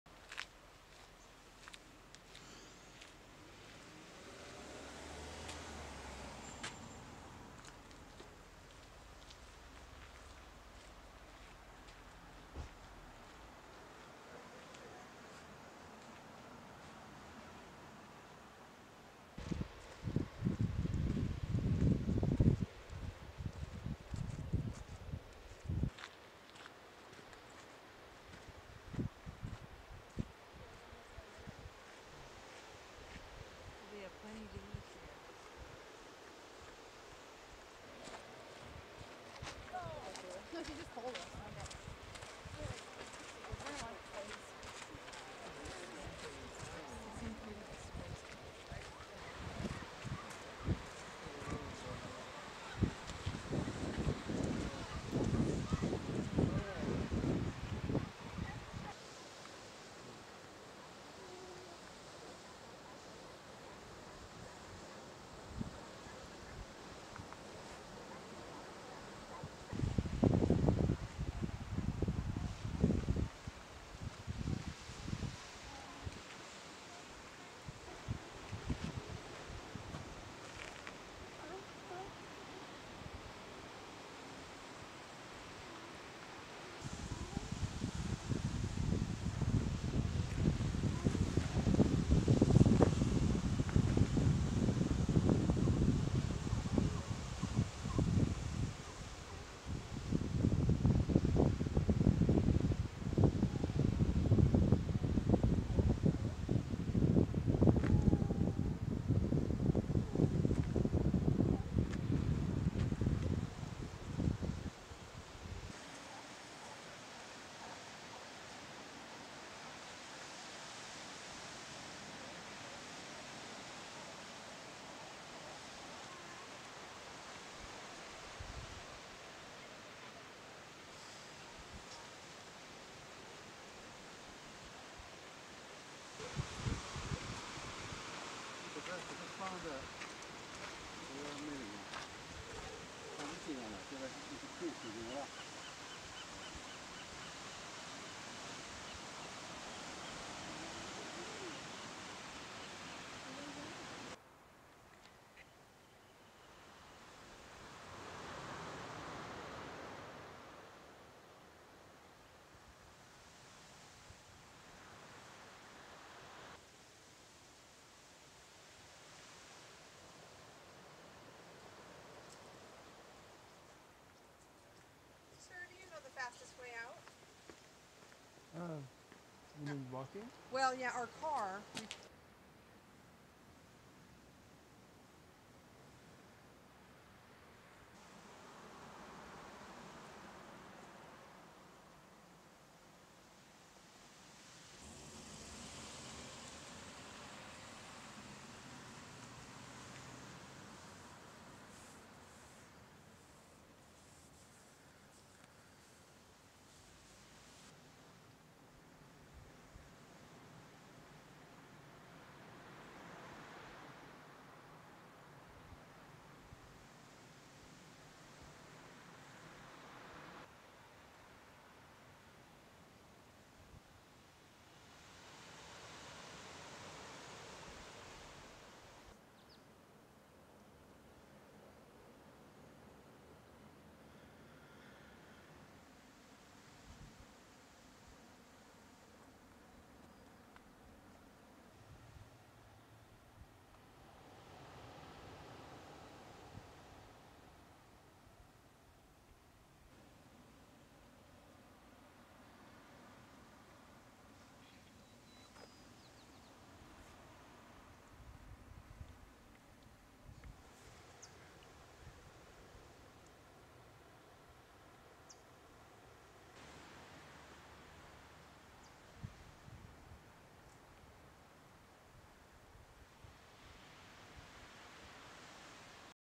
Point Lobos, California Filming Day
A brief moment with Point Lobos, California's iconic beauty.